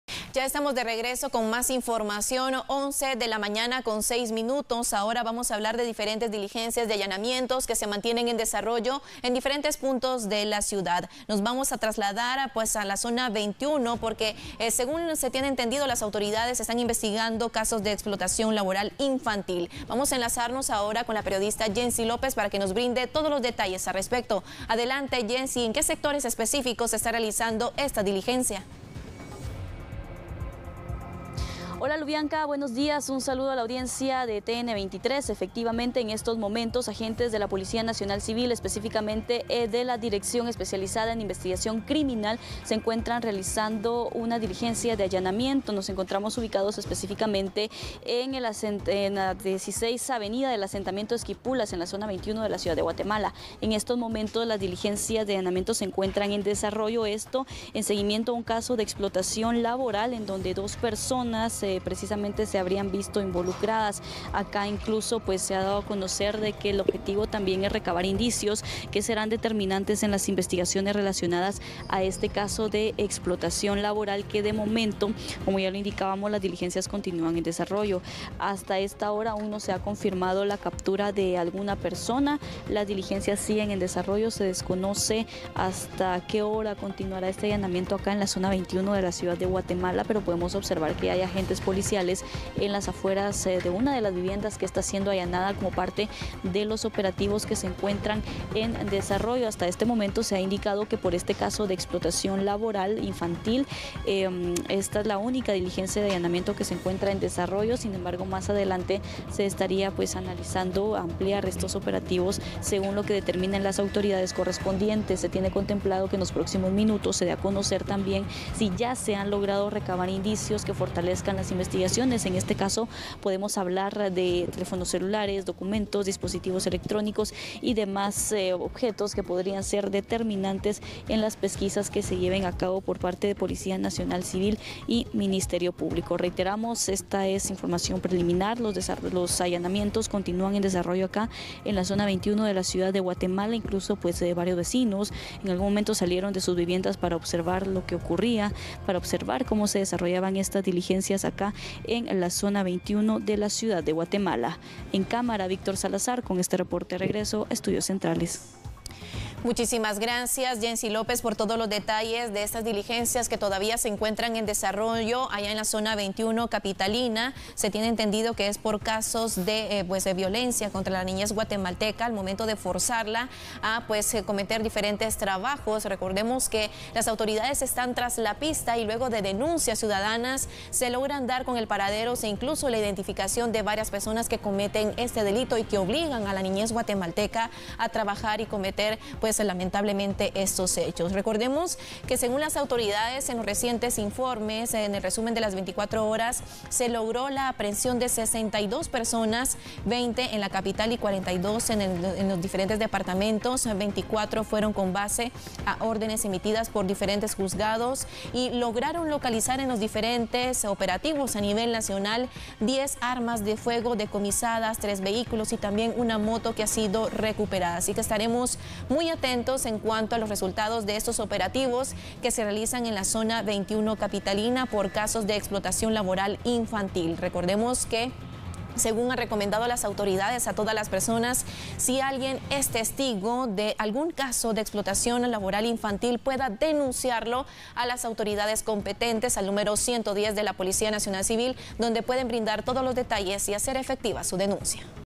PNC realiza allanamientos en zona 21 capitalina
Agentes de la Policía Nacional, específicamente de la Dirección Especializada en Investigación Criminal, realizan allanamientos en la 16° avenida del asentamiento Esquipulas, zona 21 de Ciudad de Guatemala, por un caso de explotación laboral infantil.
Hasta el momento no se ha confirmado la captura de algún sospechoso.